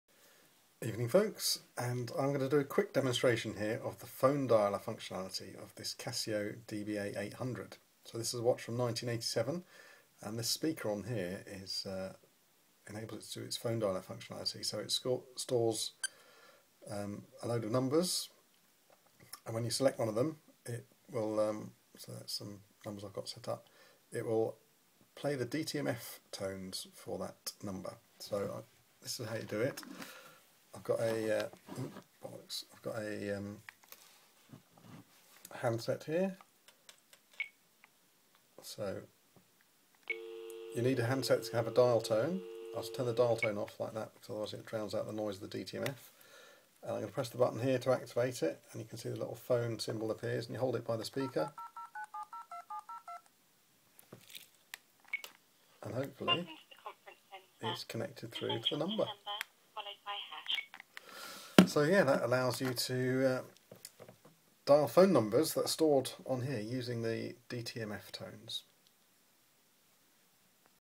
Casio Phone Dialler Quick Demo DBA-800
A short demo of this watch showing the Phone Dialler working